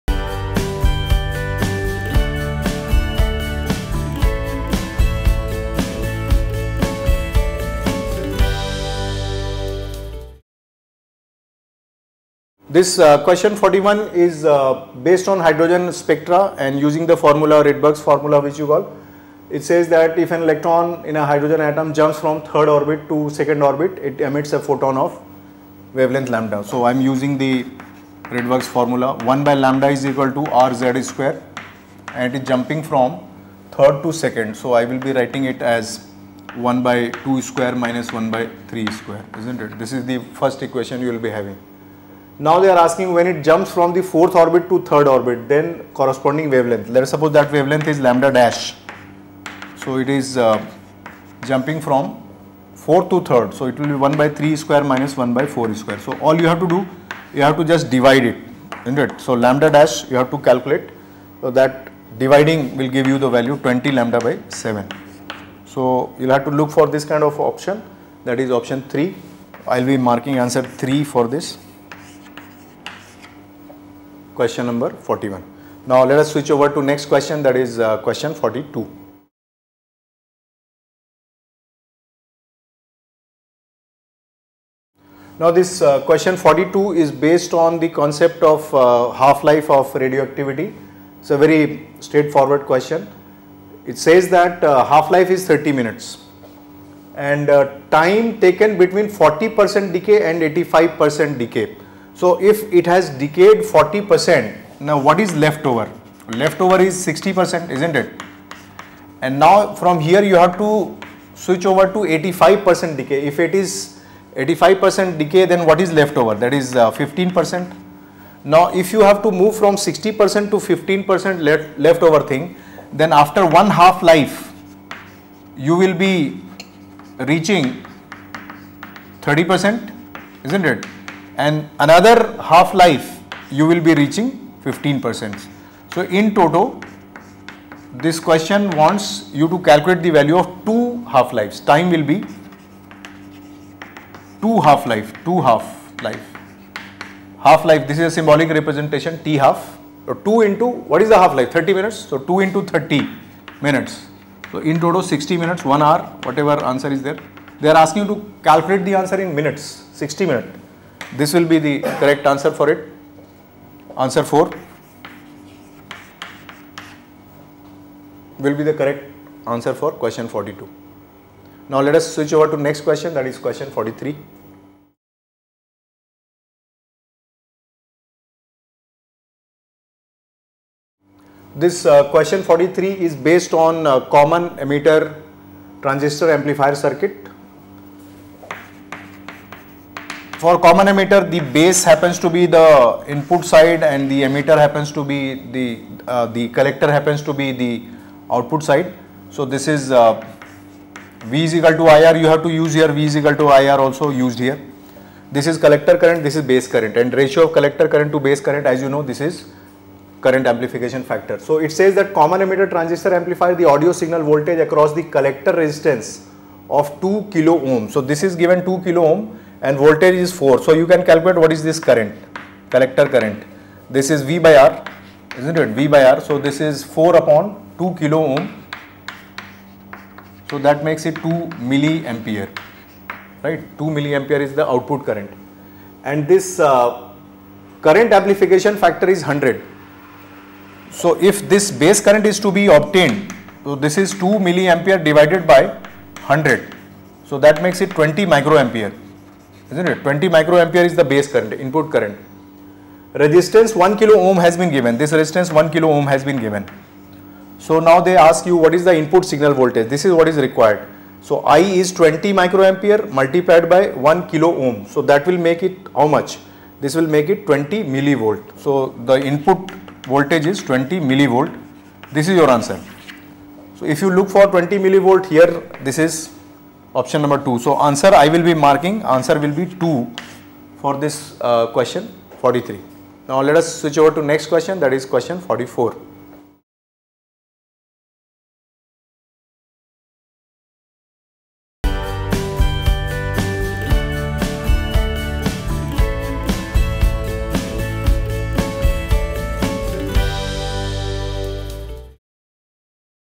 NEET-UG Phase 2 2016 - Video Solution for Physics questions 41 to 43
The video deals with question numbers 41, 42, and 43 of the NEET 2 physics paper. Question 41 is based on hydrogen-spectra. Aakash institute’s teacher uses the variables given directly in the formula of Redwing’s. Using this formula, the correct answer is derived from the question that demands to know the corresponding wavelength of the photon. Question 42 is based on the half-life of radioactivity. The question states the time in which the substance has decayed between 40% and 85%. The question demands to know the time taken in this case. Aakash deals with it by using the base concept of the half-life of radioactivity. Question 43 is based on common emitter transistor amplifier circuit, as the question demands to know the amplification factor the ratio between the base current and collector current is calculated.